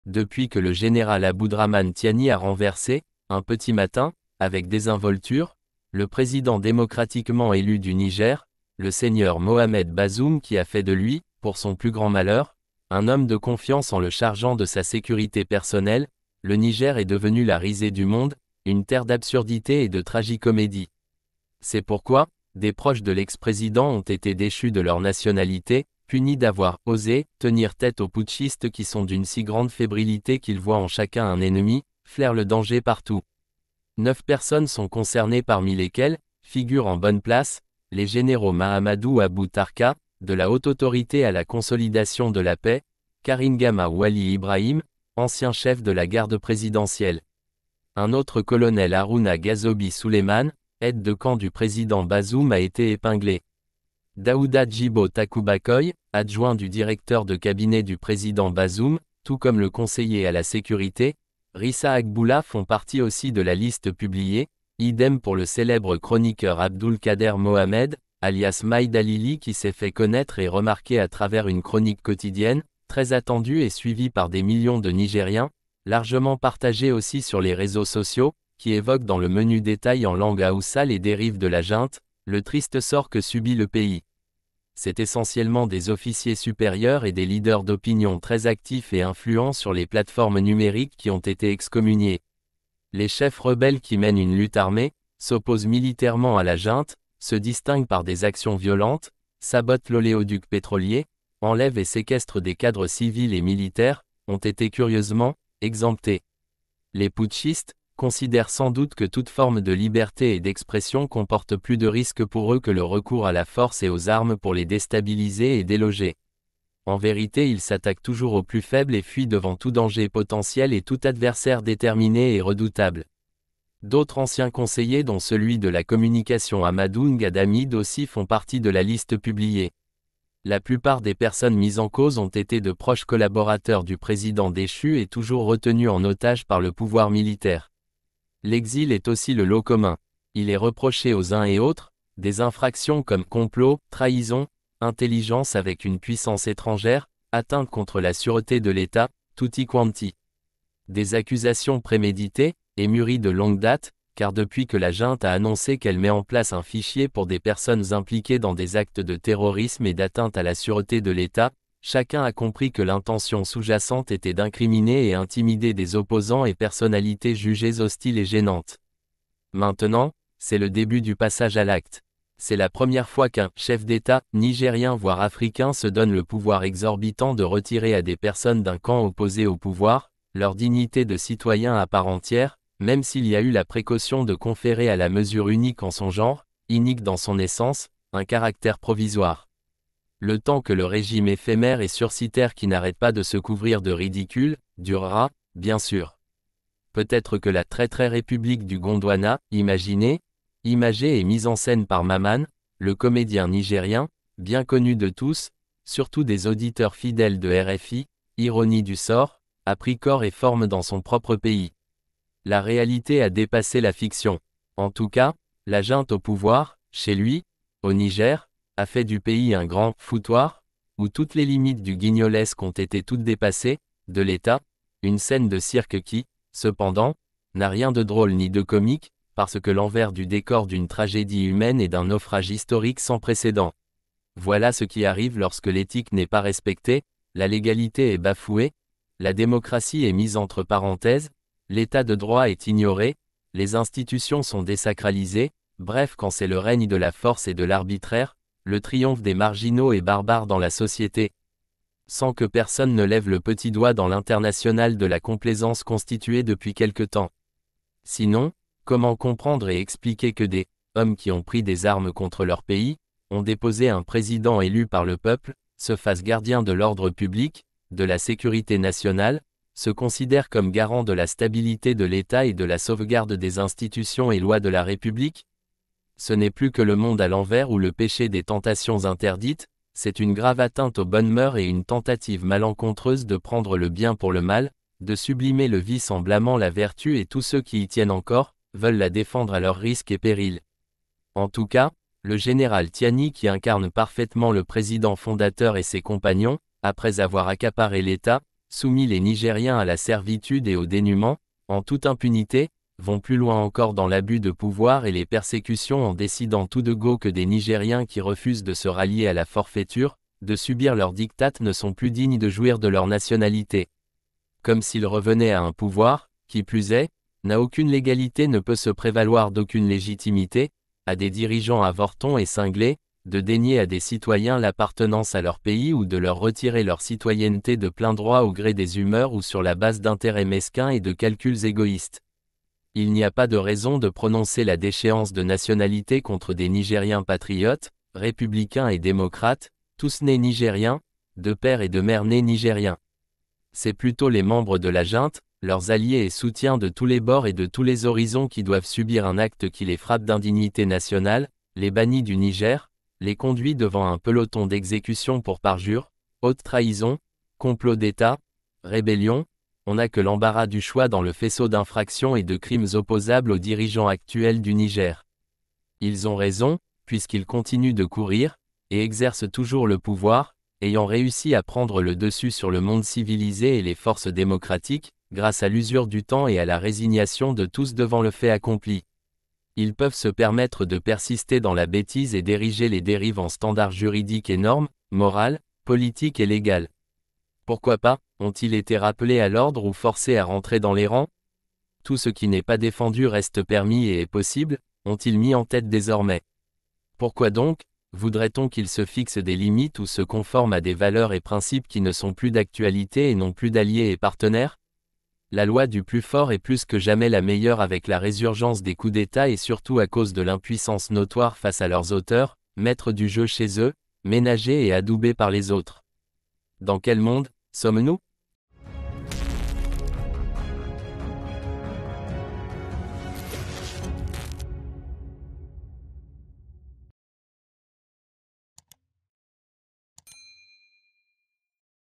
🚨C'EST INCROYABLE CE QUE GÉNÉRAL TIANI A FAIT AUX PROCHES DE MOHAMED BAZOUM. ÉCOUTEZ
🚨LES PROCHES DE L’EX-PRÉSIDENT MOHAMED BAZOUM ONT ÉTÉ DÉCHUS DE LEUR NATIONALITÉ
Pour visiter nos pages sociales :